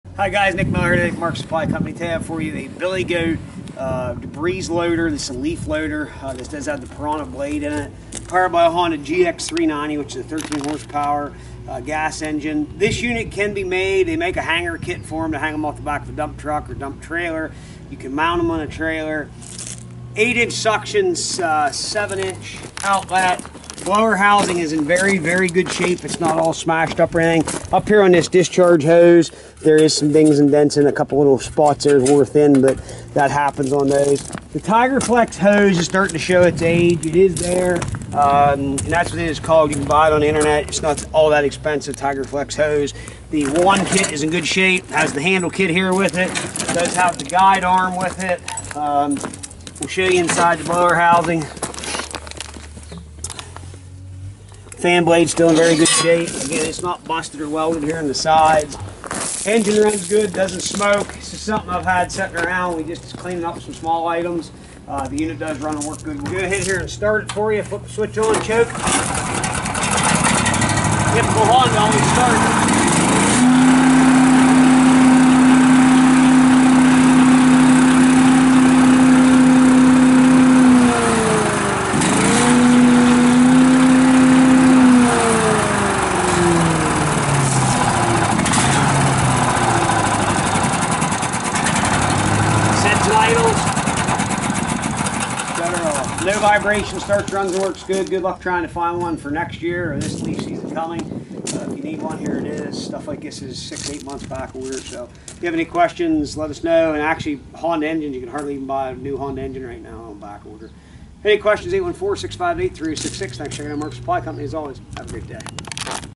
Billy Goat DL1302H Debris Loader For Truck Or Dump Trailer Leaf Loader Honda 13HP For Sale Nice!
Nice Billy Goat DL1302H 13HP Honda Debris Loader Leaf Loader For Truck Or Dump Trailer For Sale Call Mark Nick Or Matt (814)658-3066